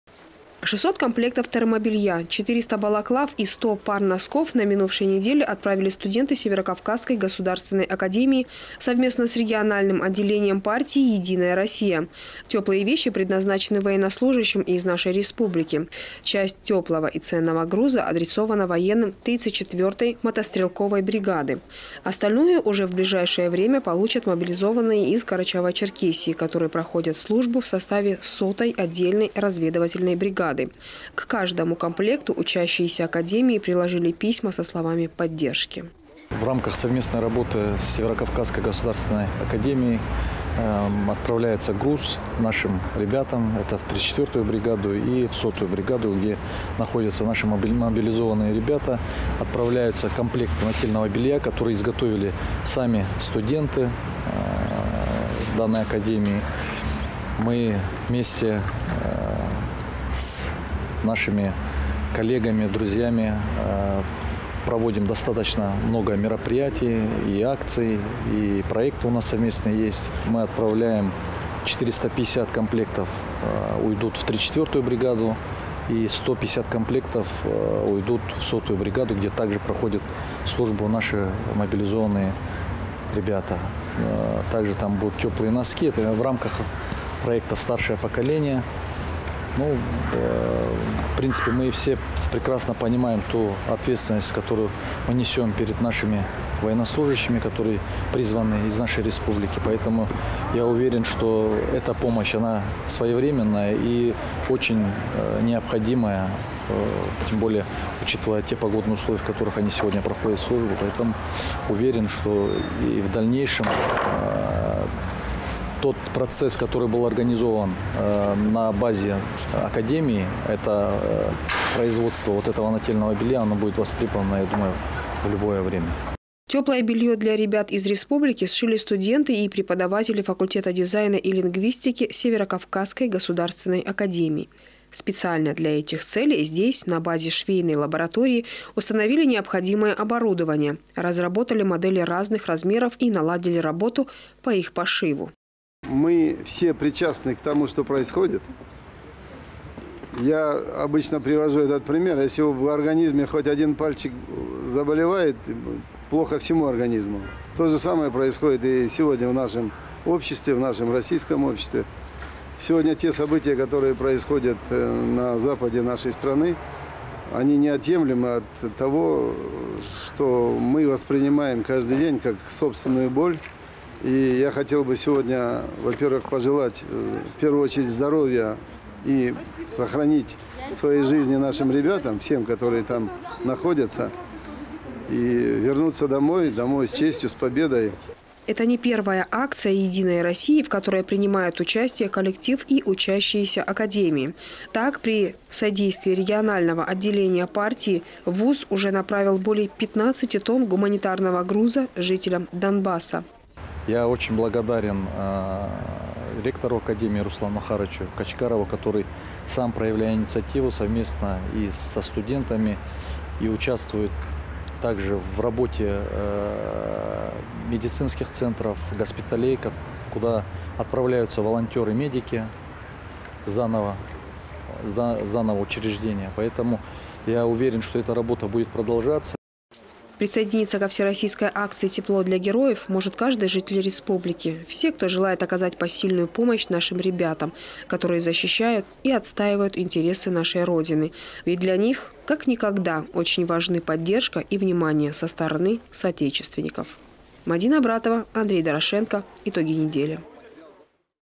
Карачаево-Черкесия присоединилась к акции «Тепло для героя»
С 10 января стартовала Всероссийская акция «Тепло для Героя» организованная партийным проектом «Старшее поколение» Партии «Единая Россия». Масштабная акция направлена в поддержку Вооруженных Сил Российской Федерации и наших доблестных солдат, которые находятся в зоне ведения специальной военной операции. К акции присоединилась и Карачаево-Черкесия.